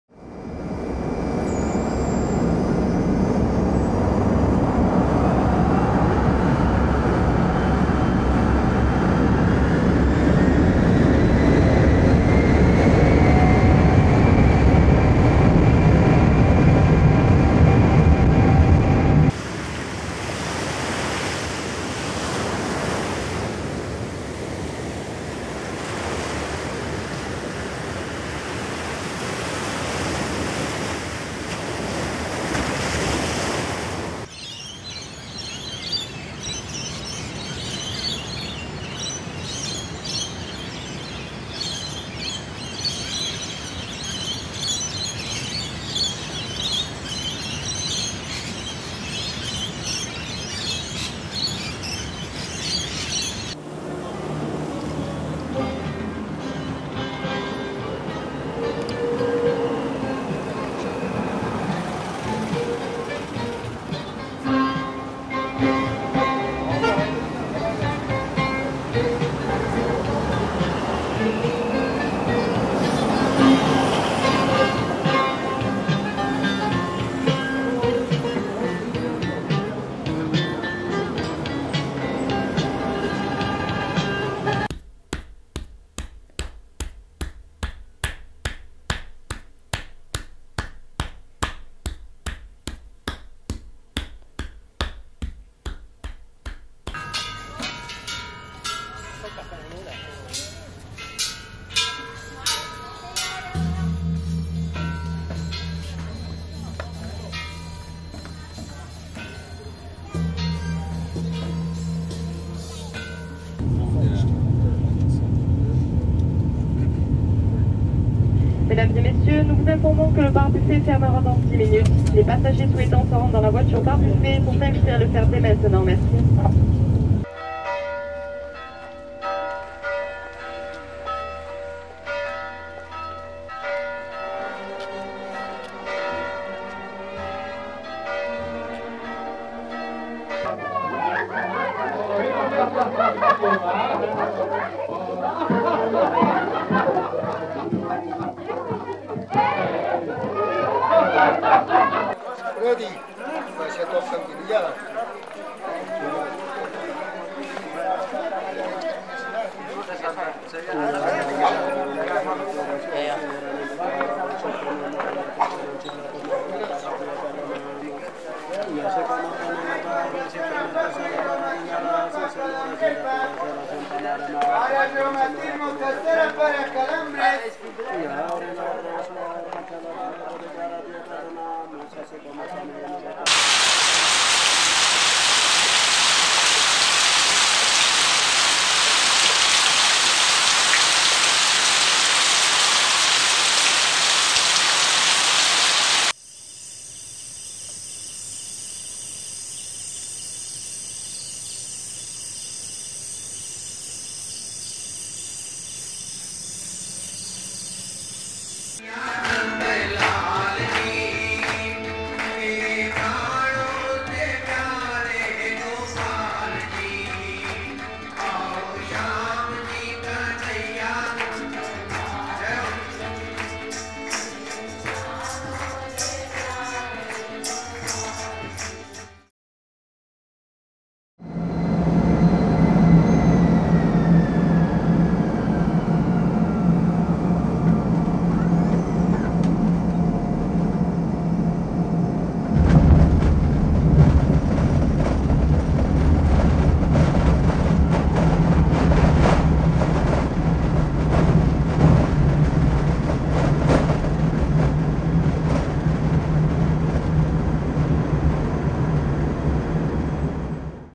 Study 18: Dreaming of Travel While on Lockdown (2020)
4 min, 21 s — USA | Part of the Life-world Series project
— 747 jet aircraft – interior. Taxi to take off – undercarriage raised at 2’00, indicator board gong – aircraft climbs.
— Surf – Bondi beach.
— Lorikeets at dawn, Manly beach, Australia – raccous dawn chorus of Rainbow Lorikeets in tall trees against a background of Tasman sea surf.
— Istanbul, street music, with passing traffic and voices.
— Chinese Buddhist Temple, exterior, atmosphere with gongs, visitors walking in grounds & tapping wishing bells.
— Thumping massage.
— Train travelling in France with announcements at 1’22” (First class compartment).
— Asian Jungle night-time atmosphere.
— Storm recorded on Solomon Islands.
— Church bells and village band outside church (Poland) during harvest festival celebrations, bells, chat, brass band (very bad) makes circuit of church playing hymn and returns (indexed 2’02”).
— Russian Crowd, mixed, happy.
— Street, Cliza, on market day with cries from hawkers, footsteps and bells and chanting from blind fortune tellers, no traffic.
— Singing in temple leader on PA, congregation singing, clapping and playing bells
747 jet aircraft – interior. Descent – touch down – taxi.